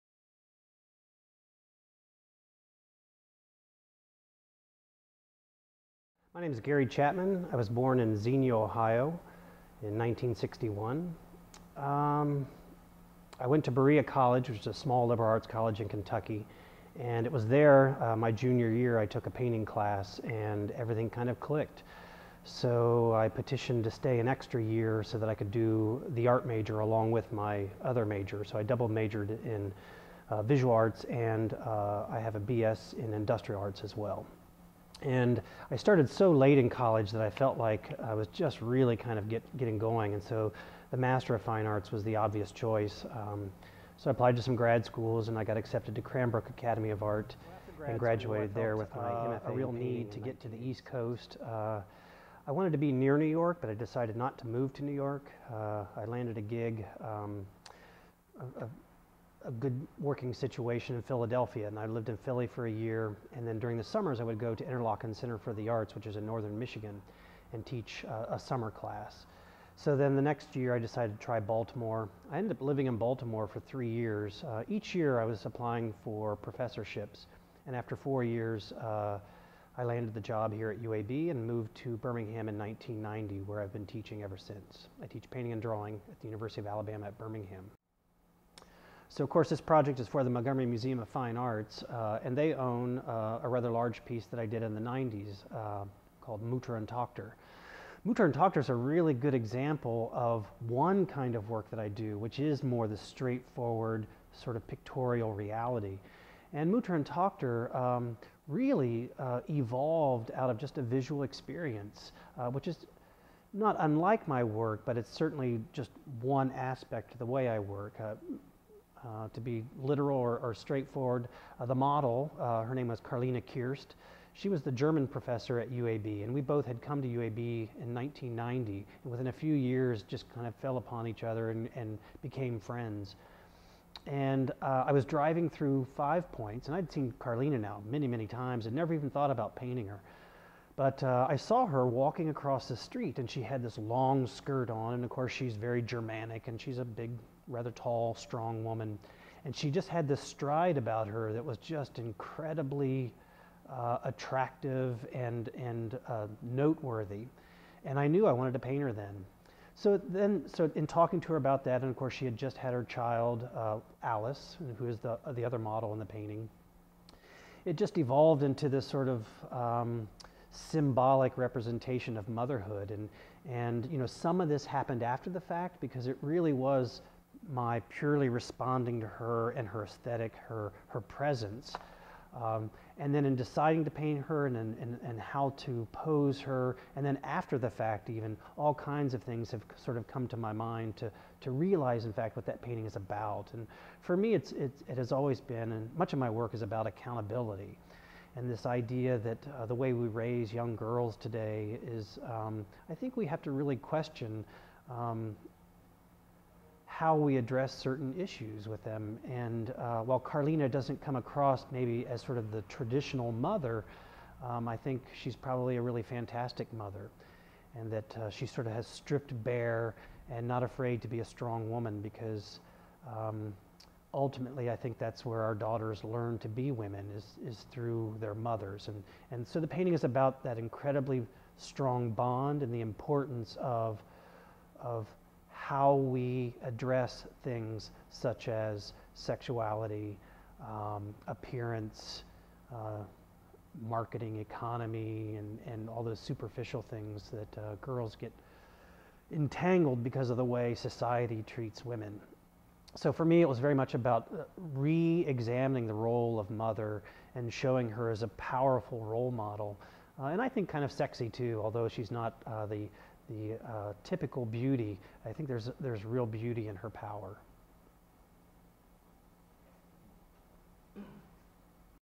Gary Chapman: An Introduction
Gary Chapman is an artist whose work is featured in the Museum's permanent collection. This video is part one of three videos that explore his life and work.